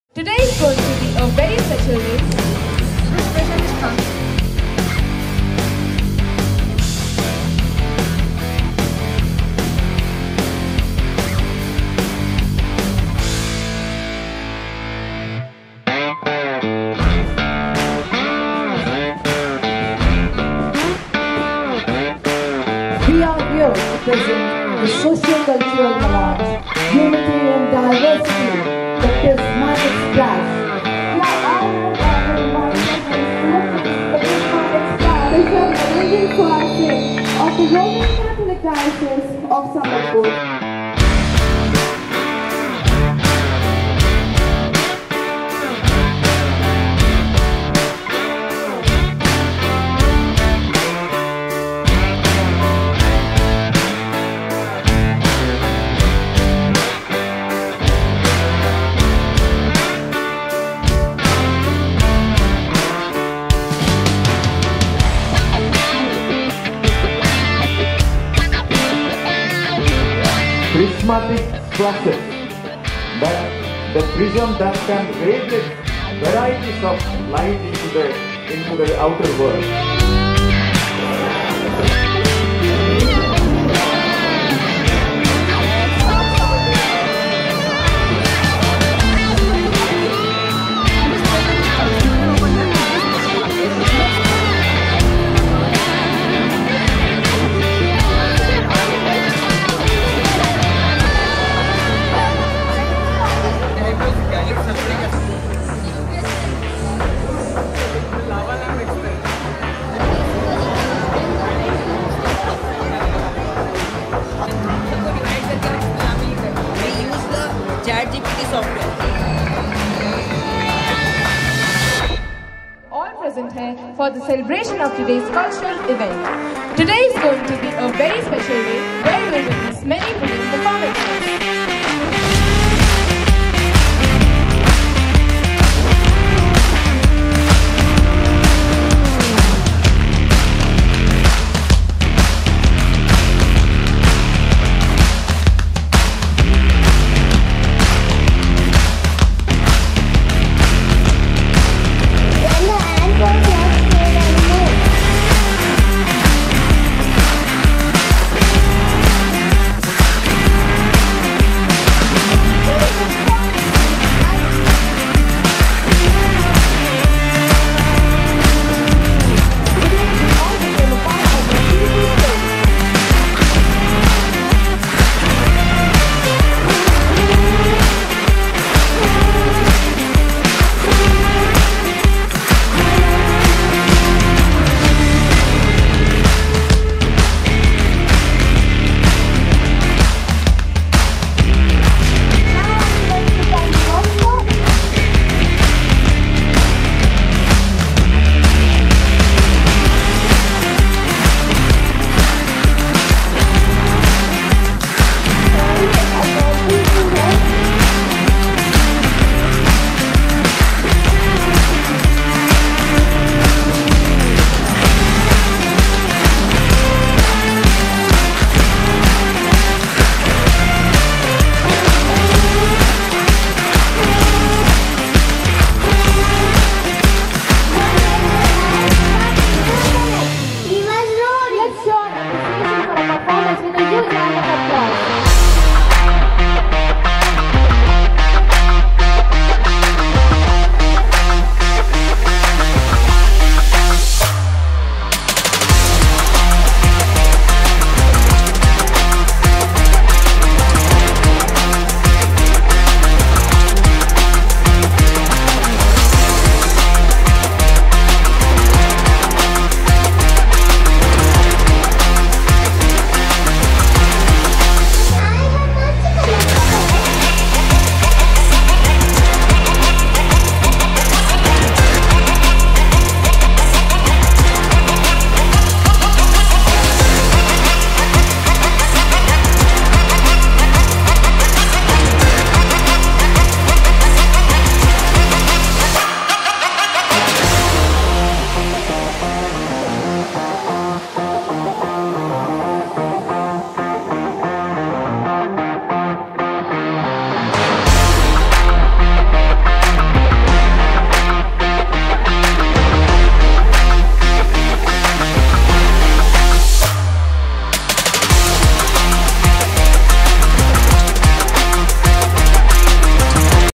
PRISMATIC SPLASH
Immerse yourself in the kaleidoscopic spectacle of evolving scenes, vibrant hues, and dynamic performances that showcase the boundless creativity and talent of our students. Prepare yourself to be captivated by the extraordinary display of artistic ingenuity as we unveil a panorama of changing patterns and breathtaking expressions.